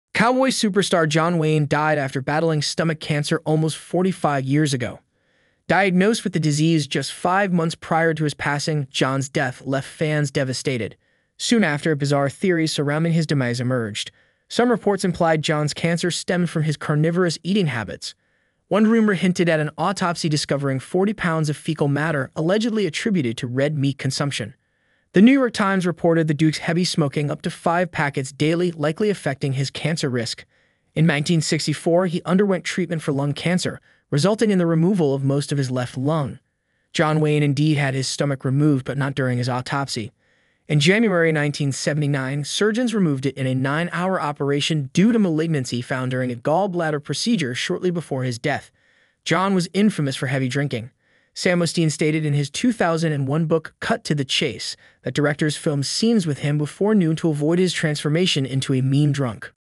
John Wayne's bizarre autopsy rumors
Cowboy superstar John Wayne died after battling stomach cancer almost 45 years ago. Diagnosed with the disease just five months prior to his passing, John's death left fans devastated. Soon after, bizarre theories surrounding his demise emerged.